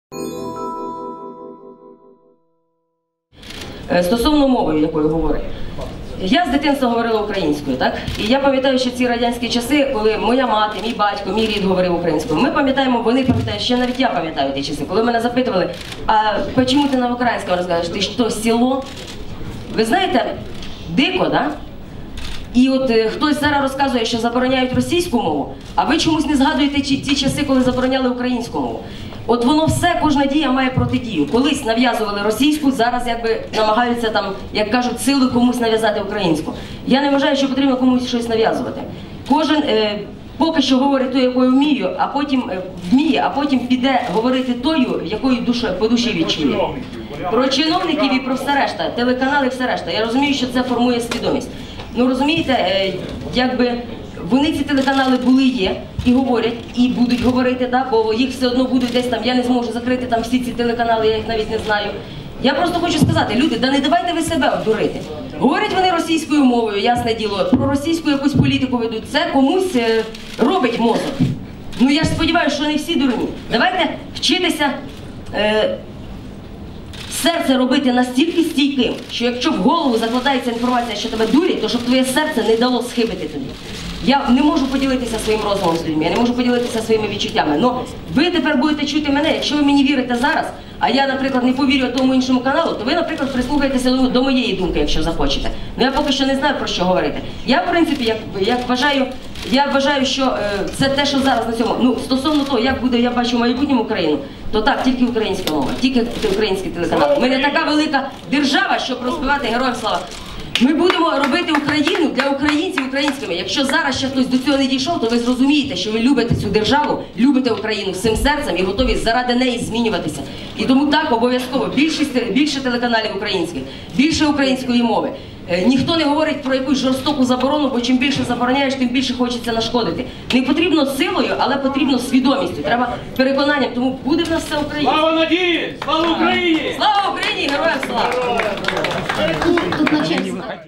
Надія Савченко про українську мову
Народний депутат України Надія Савченко про українську мову під час своєї першої прес-конференції в палаці "Україна"

Відео: Gazeta.ua